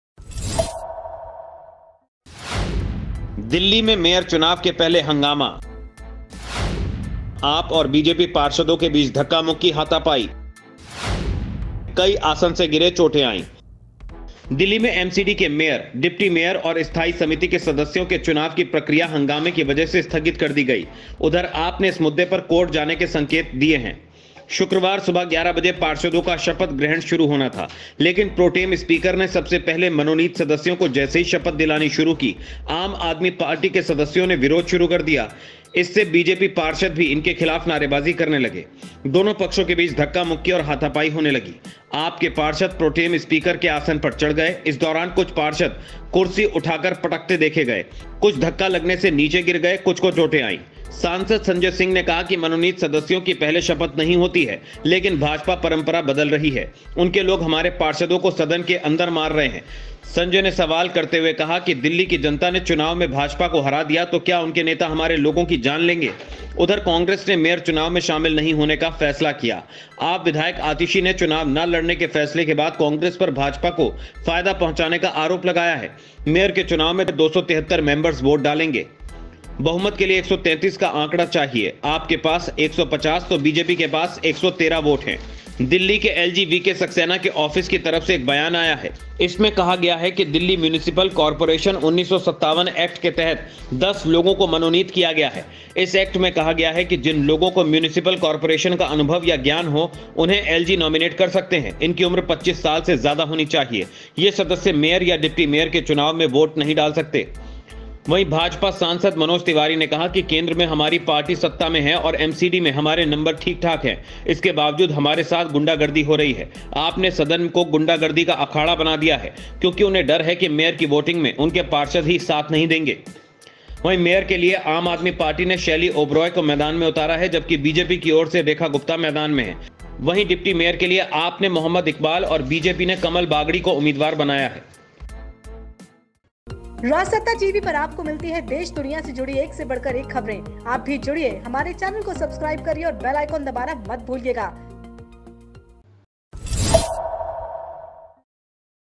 MCD Mayor Election LIVE: चुनाव से पहले भारी हंगामा, जमकर चले लात-घूंसे ! AAP Vs BJP
AAP पार्षदों ने जमकर हंगामा किया. इस दौरान आप पार्षदों के साथ बीजेपी पार्षदों की हाथापाई भी हुई. दोनों पार्टियों के पार्षदों में जमकर धक्का मुक्की हुई. यहां तक कि सदन में कुर्सियां भी चलीं. आप पार्षद मनोनीत सदस्यों को पहले शपथ दिलाने का विरोध कर रहे हैं. इससे पहले आम आदमी पार्टी ने एलजी द्वारा मेयर चुनाव के लिए बीजेपी की पार्षद सत्या शर्मा को पीठासीन अधिकारी नियुक्त करने पर आपत्ति जताई. उधर, कांग्रेस ने इस चुनाव में वोटिंग में हिस्सा न लेने का फैसला किया है.